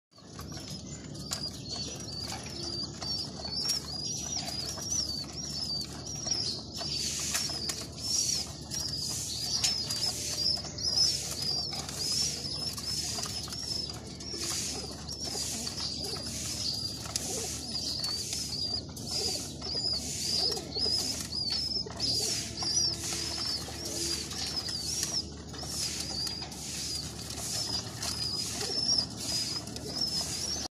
লক্ষা কবুতরের খাবার
lokkha pigeon food allwes
Clear food . Clear food pinons healthily. you cane mixture varies tipe food,লক্ষা কবুতরের খাবার What is the favorite food of pigeon like a rega Corn paddy, Wheat, Mustard, & Etc .